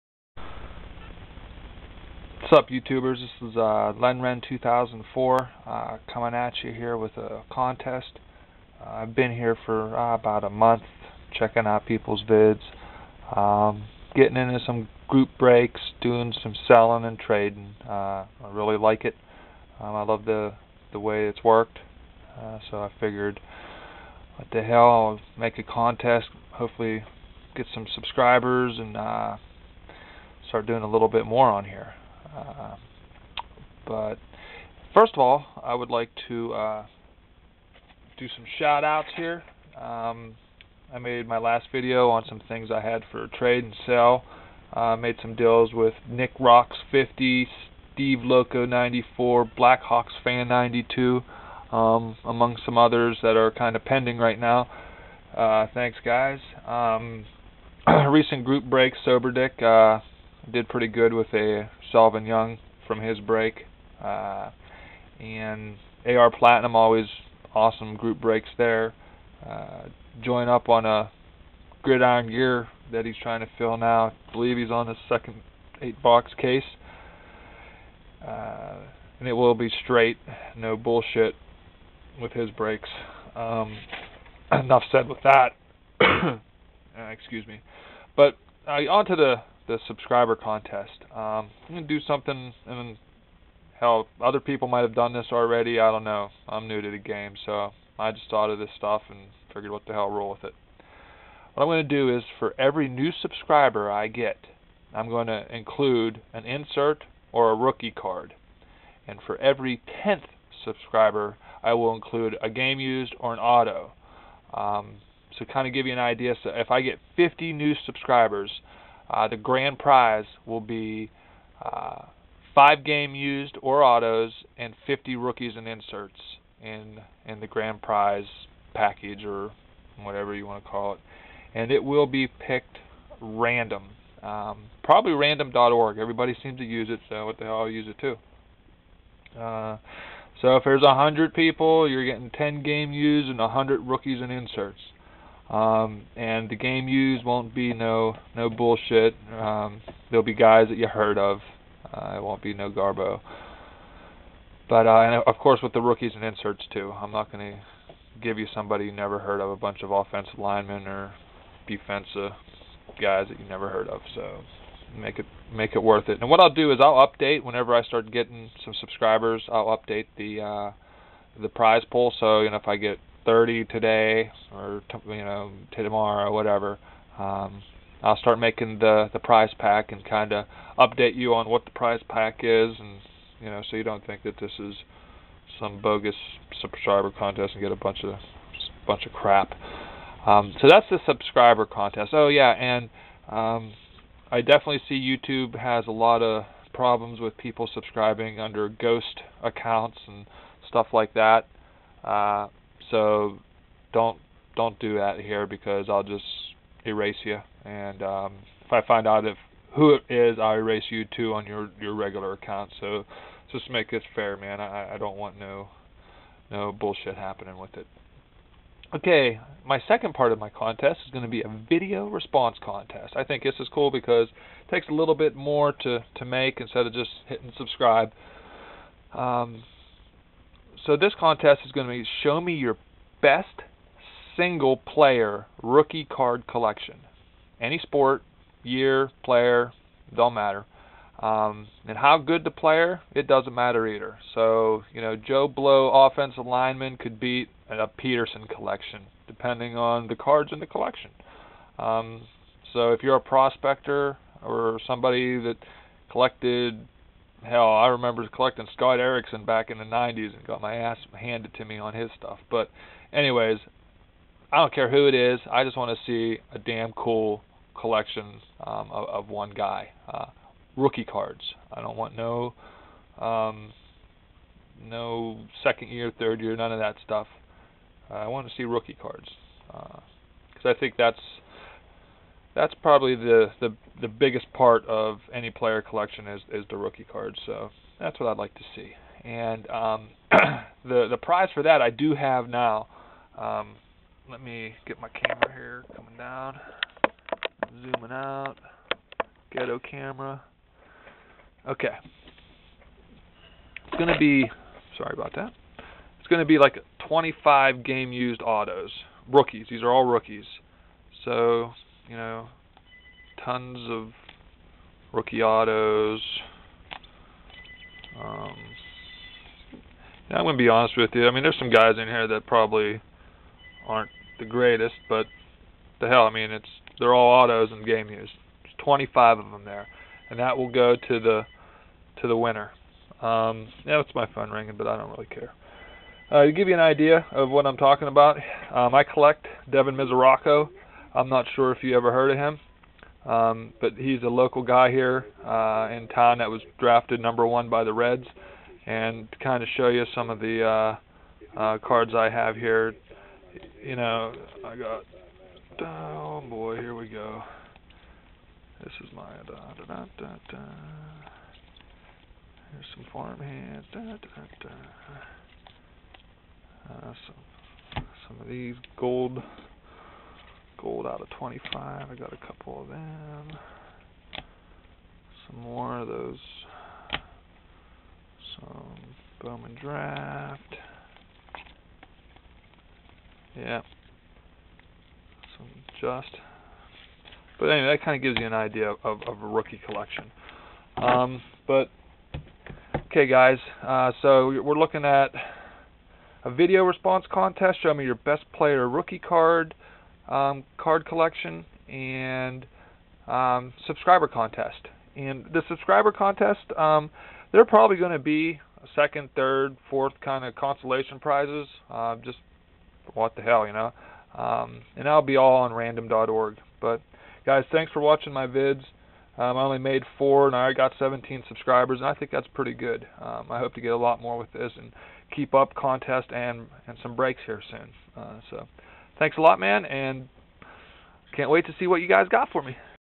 Free Contest For Baseball Football Hockey Basketball Cards
This is a free contest to win baseball, football, hockey and basketball cards. Checkout the video and good luck!